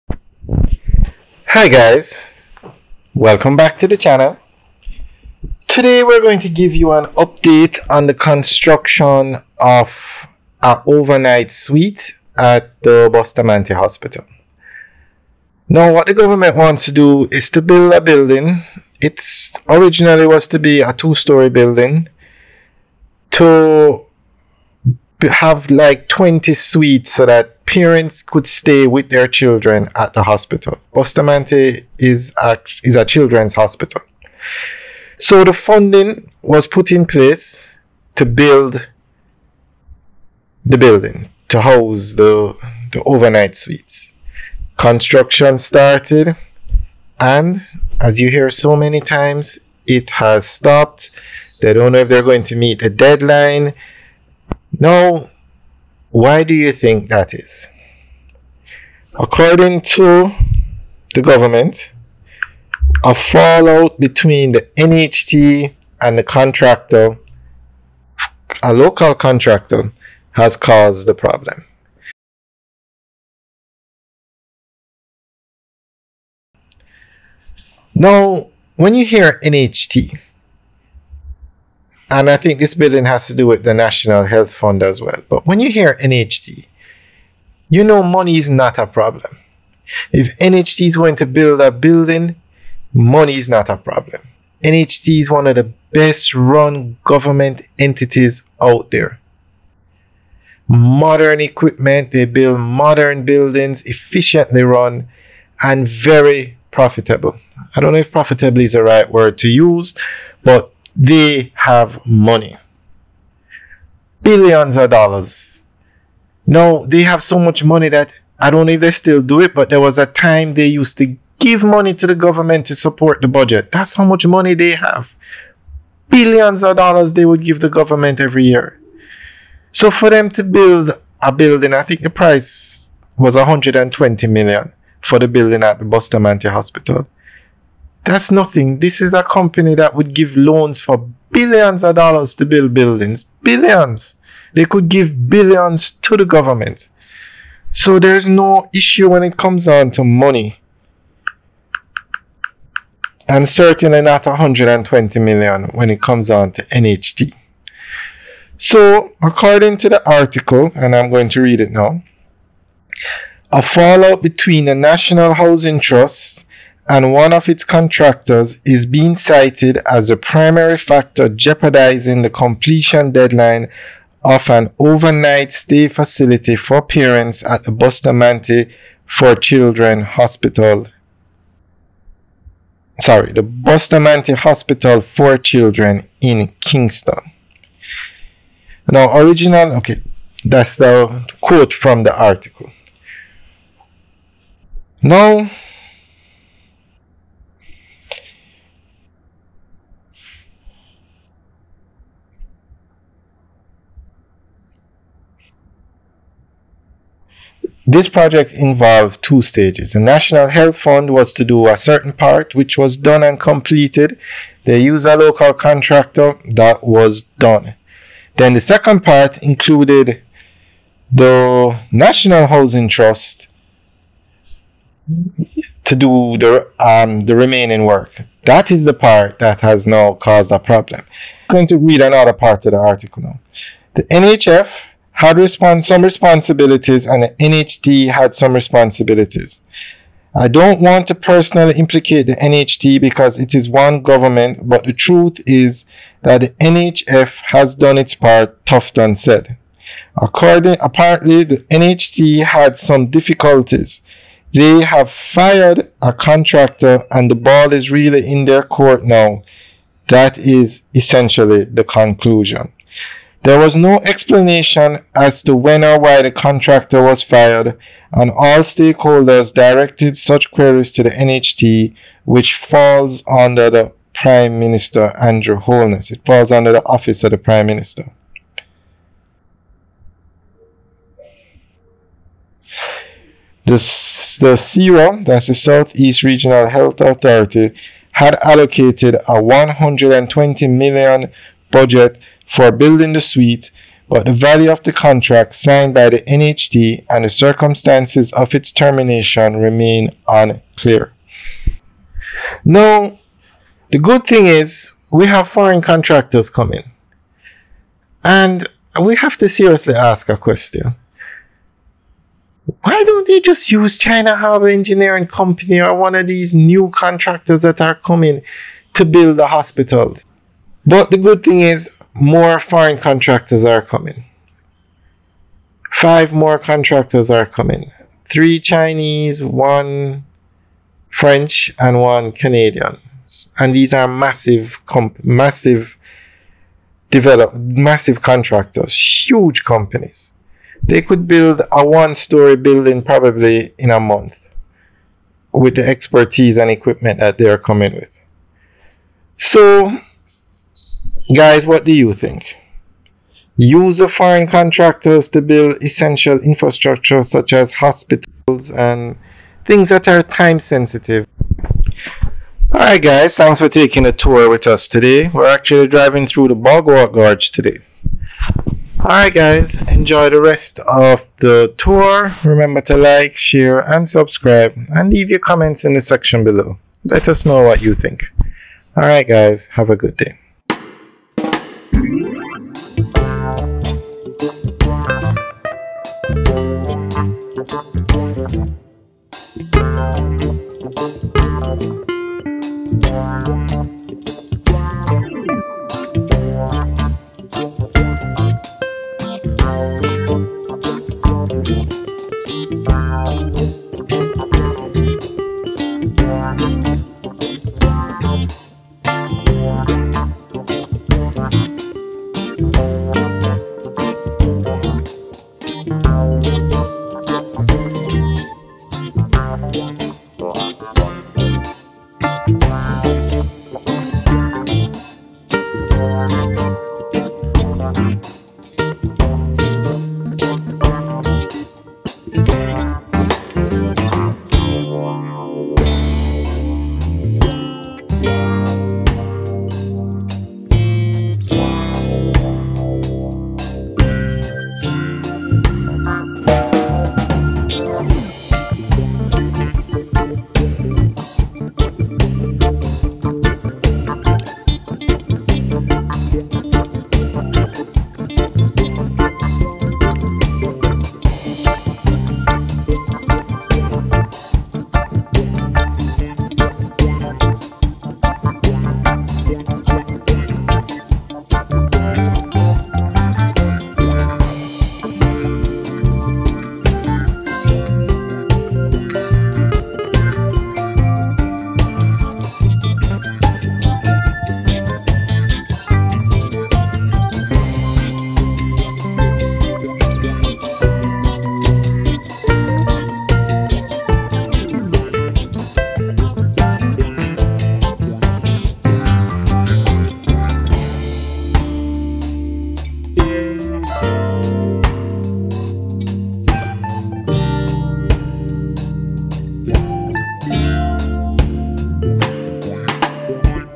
Local Contractor Fired Construction Bustamante Hospital Jamaica
A narrated driving tour Local Contractor Fired Construction Bustamante Children's Hospital Jamaica